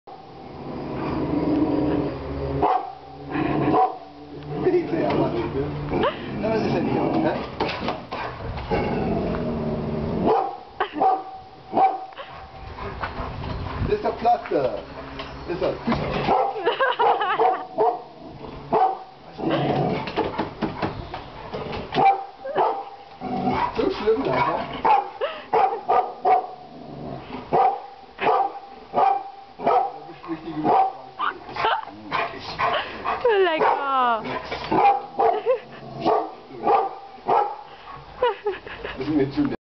dangerous rhinoceros and a brave dog
Das ist meine Hündin Laica im "Schweriner Zoo"!
Dieses Nashorn ist aus Kunststoff und Laica wollte nicht an ihm vorbeigehen... am Ende sind wir zurück in die Richtung gegangen aus der wir gekommen sind.